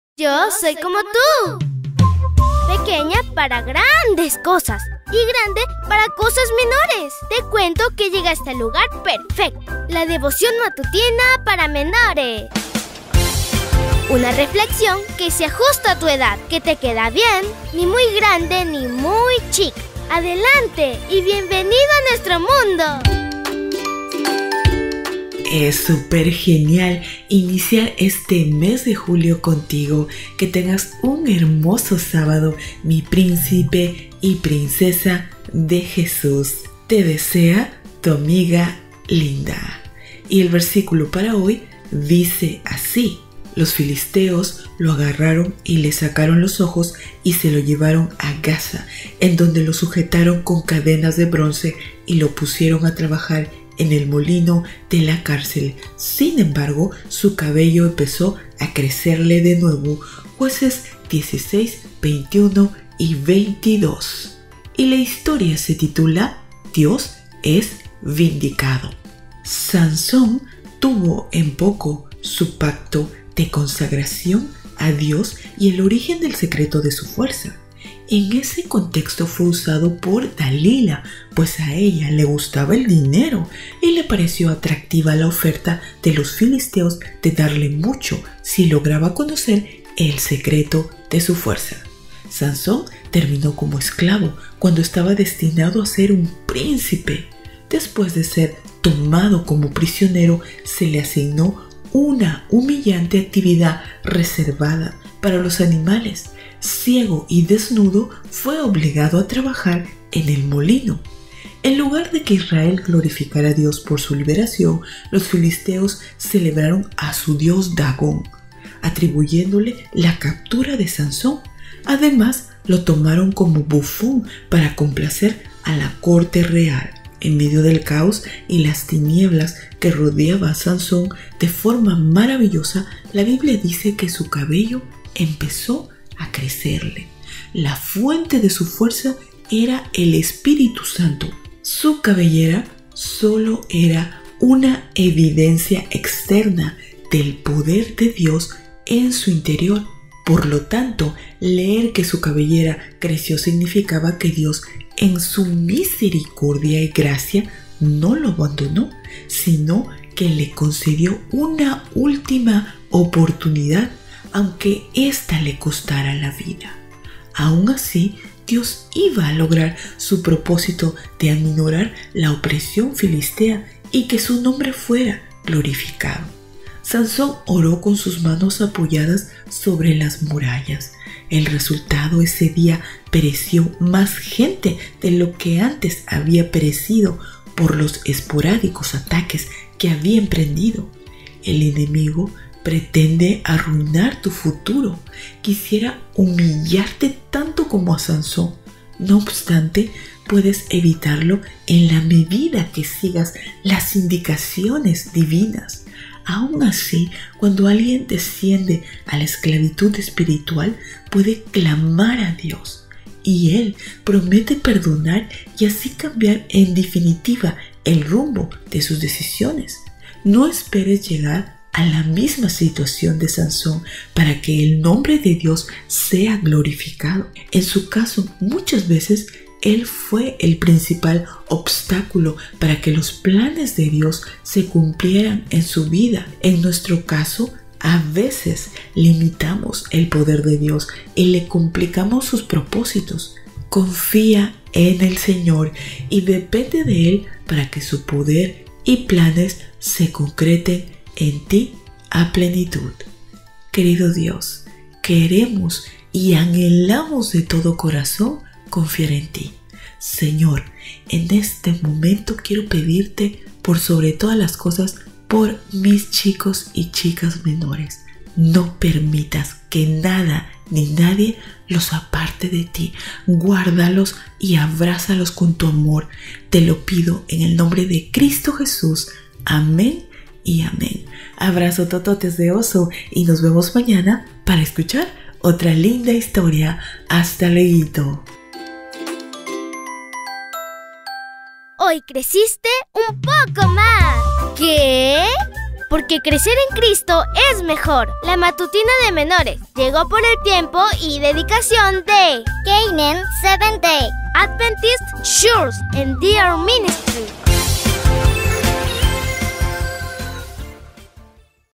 Devoción Matutina Para Menores - 01 de Julio del 2023
01 DE JULIO
DIOS ES VINDICADO

«Los filisteos lo agarraron y le sacaron los ojos, y se lo llevaron a Gaza, en donde lo sujetaron con cadenas de bronce y lo pusieron a trabajar en el molino de la cárcel. Sin embargo, su cabello empezó a crecerle de nuevo» (Jueces 16:21-22).

Sansón tuvo en poco su pacto de consagración a Dios y el origen del secreto de su fuerza. En ese contexto fue usado por Dalila, pues a ella le gustaba el dinero y le pareció atractiva la oferta de los filisteos de darle mucho si lograba conocer el secreto de su fuerza. Sansón terminó como esclavo cuando estaba destinado a ser un príncipe. Después de ser tomado como prisionero, se le asignó una humillante actividad reservada para los animales; ciego y desnudo fue obligado a trabajar en el molino. En lugar de que Israel glorificara a Dios por su liberación, los filisteos celebraron a su dios Dagón (vers, 23-24) atribuyéndole la captura de Sansón. Además, lo tomaron como bufón para complacer a la corte real.
En medio del caos y las tinieblas que rodeaban a Sansón, de forma maravillosa la Biblia dice que «su cabello empezó a crecerle». La fuente de su fuerza era el Espíritu Santo, su cabellera solo era una evidencia externa del poder de Dios en su interior. Por lo tanto, leer que su cabellera creció significa que Dios en su misericordia y gracia no lo abandonó, sino que le concedió una última oportunidad, aunque esta le costara la vida. Aun así, Dios iba a lograr su propósito de aminorar la opresión filistea y que su nombre fuera glorificado. Sansón oró con sus manos apoyadas sobre las murallas; el resultado: ese día pereció más gente de la que antes había perecido por los esporádicos ataques que había emprendido (vers, 27-30).
El enemigo pretende arruinar tu futuro, quisiera humillarte tanto como a Sansón. No obstante, puedes evitarlo en la medida que sigas las indicaciones divinas. Aun así, cuando alguien desciende a la esclavitud espiritual, puede clamar a Dios y él promete perdonar y así cambiar en definitiva el rumbo de sus decisiones. No esperes llegar a la misma situación de Sansón para que el nombre de Dios sea glorificado. En su caso, muchas veces él fue el principal obstáculo para que los planes de Dios se cumplieran en su vida. En nuestro caso, a veces limitamos el poder de Dios y le complicamos sus propósitos. Confía en el Señor y depende de él para que su poder y planes se concreten en ti a plenitud.